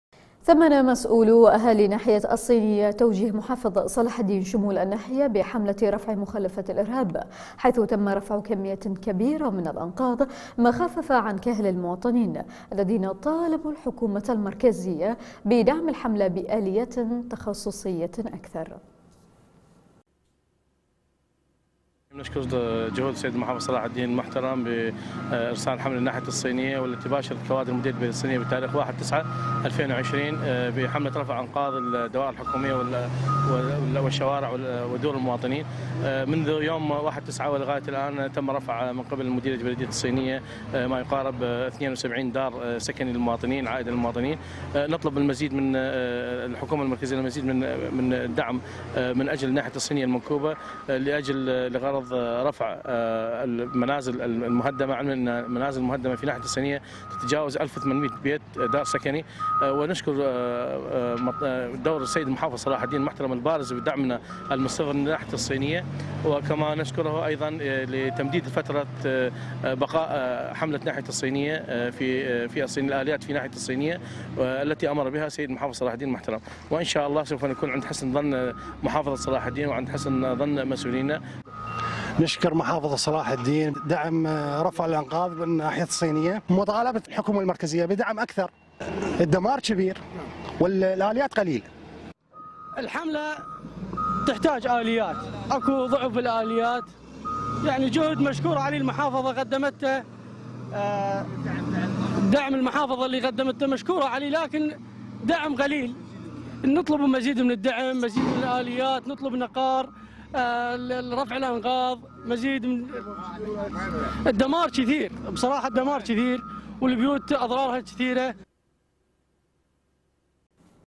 مطالبة الحكومة المركزية بدعم حملة الصينية
صلاح الدين

ثمّن مسؤولو واهالي ناحية الصينية توجيه محافظ صلاح الدين شمول الناحية بحملة رفع مخلفات الارهاب, حيث تم رفع كميات كبيرة من الانقاض ما خفف عن كاهل المواطنين الذين طالبو الحكومة المركزية بدعم الحملة بآليات تخصصية اكثر